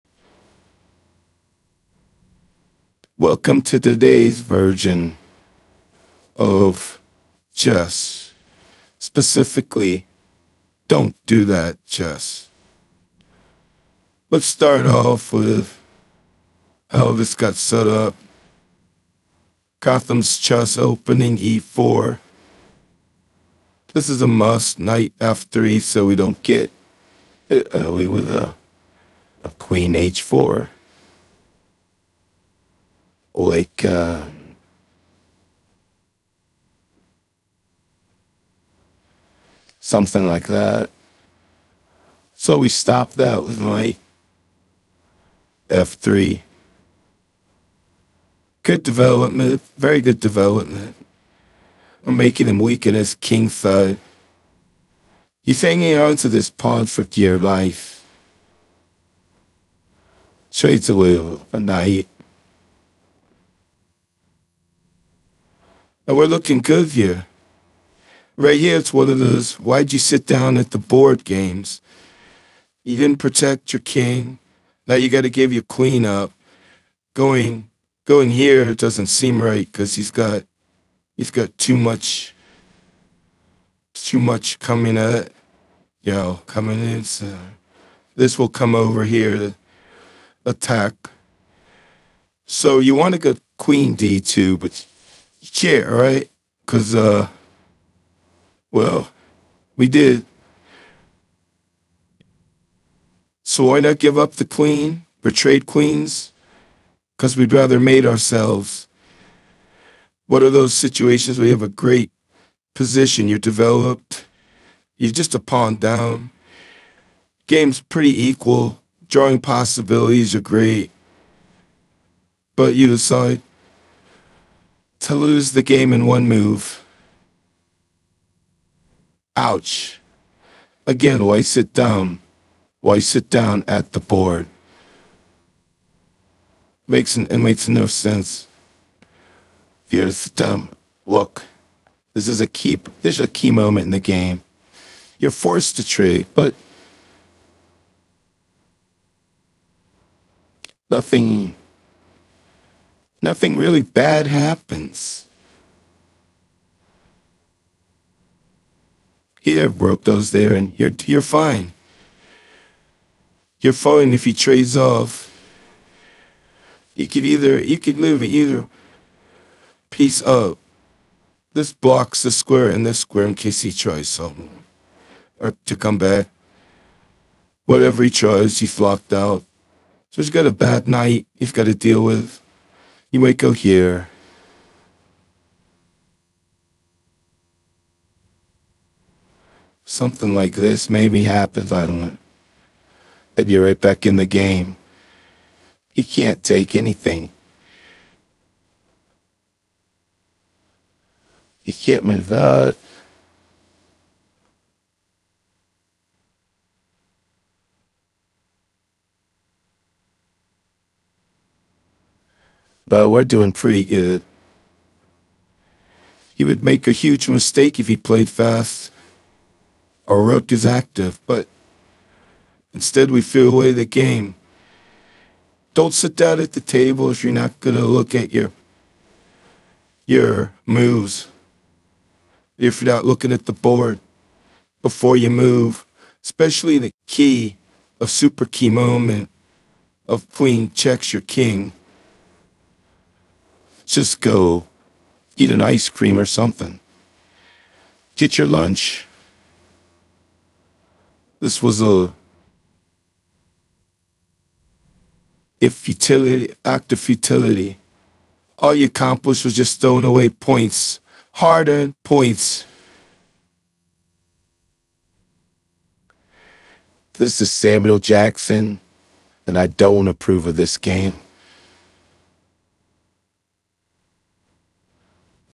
Don't do this in Chess
When you sit down and spend your time playing a chess game you want to get a good return on your time.  Here we teach a valuable lesson.